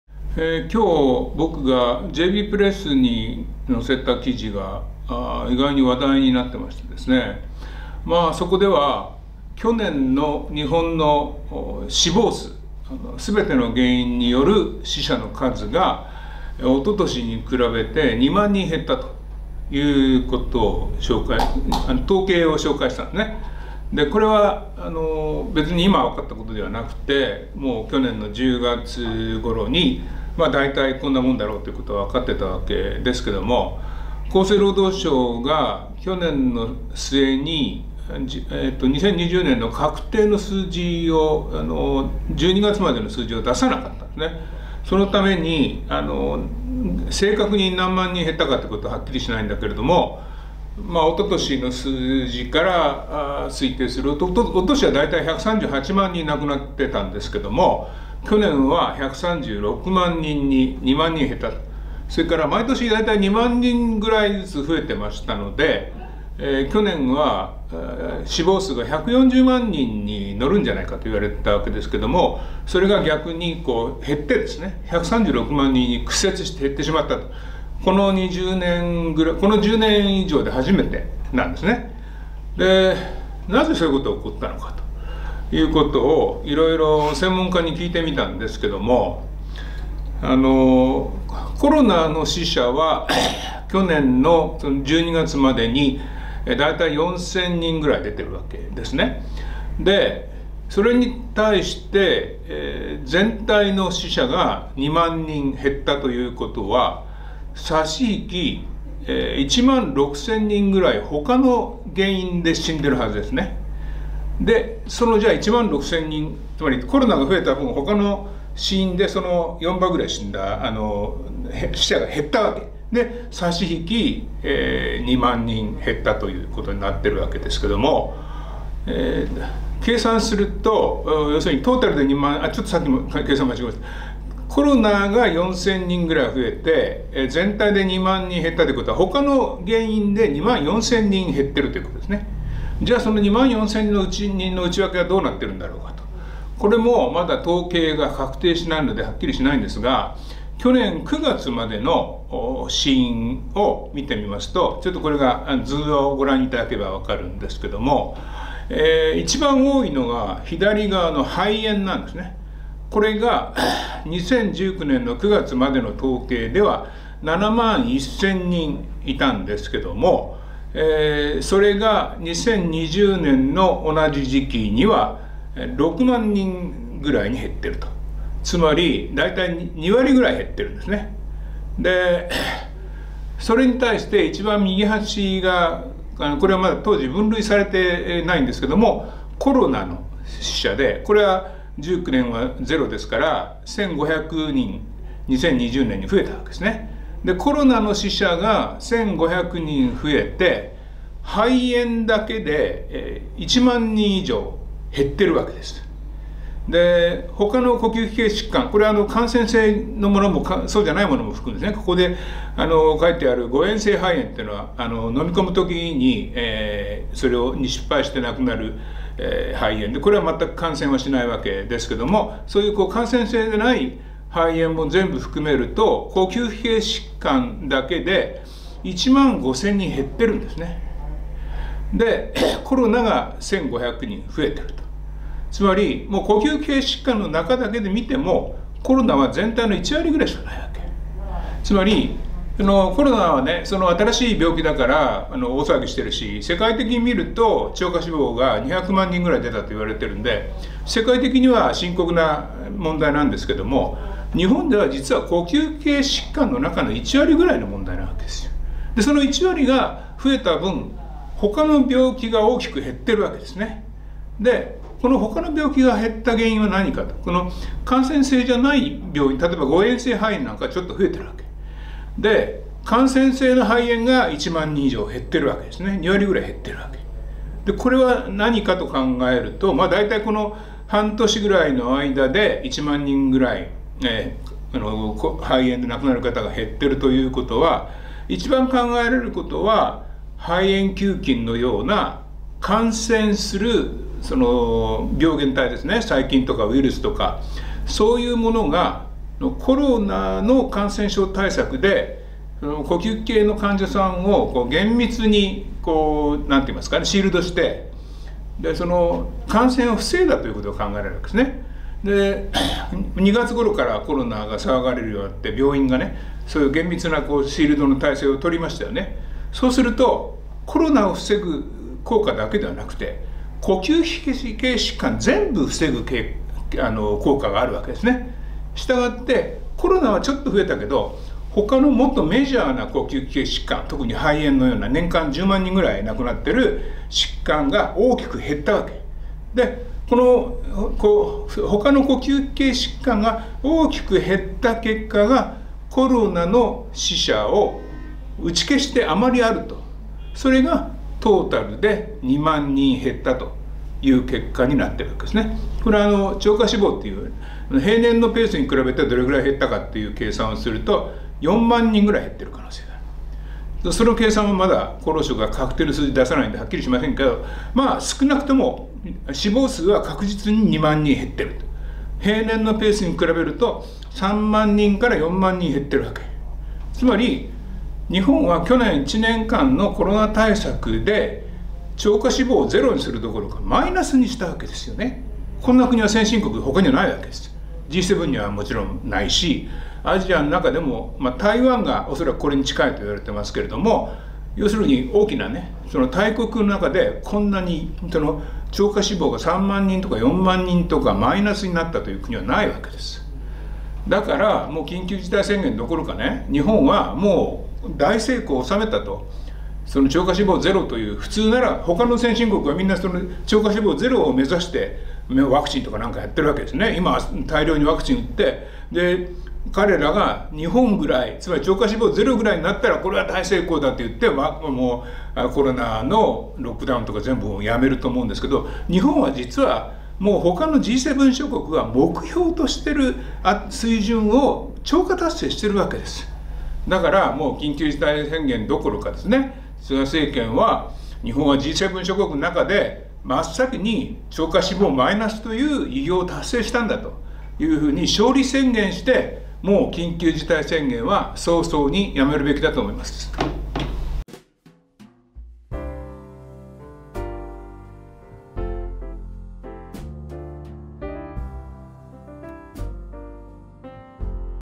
【Vlog】日本のコロナ対策は大成功
アゴラチャンネルで池田信夫のVlog、「日本のコロナ対策は大成功」を公開しました。